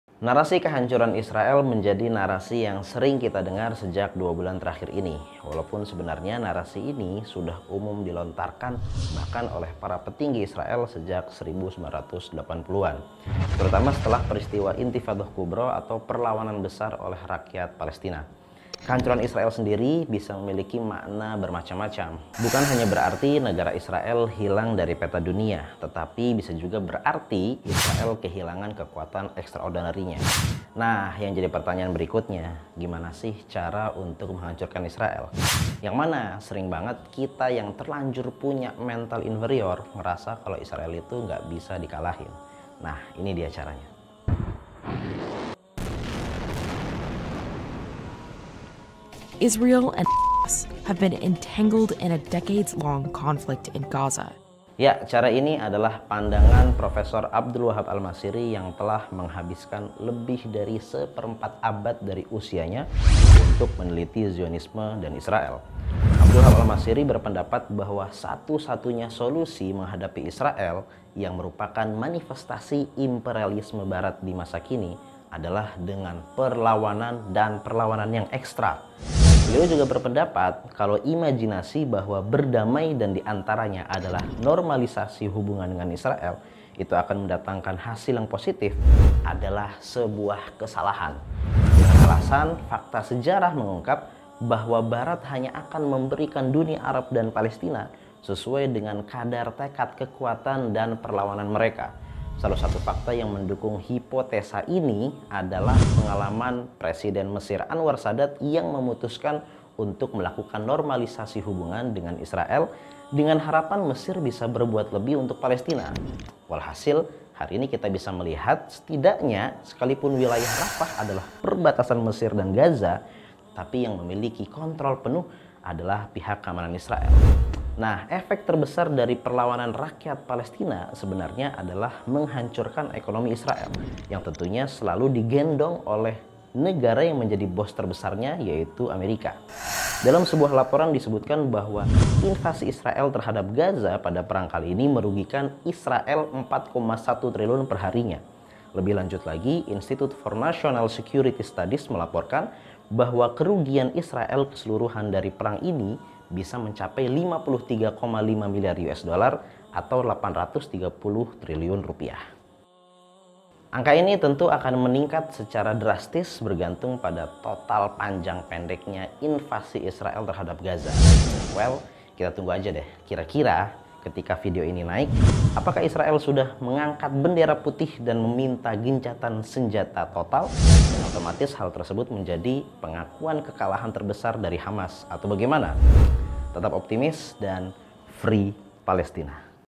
ROADMAP KEHANCURAN ISRAEL | Pengantar Konflik Palestina Episode 12
TASHLIH COMMUNITY

DONASI DAKWAH TASHLIH TV

MAYBANK SYARIAH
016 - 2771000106
a.n YAYASAN BARKA MARITIME INDONESIA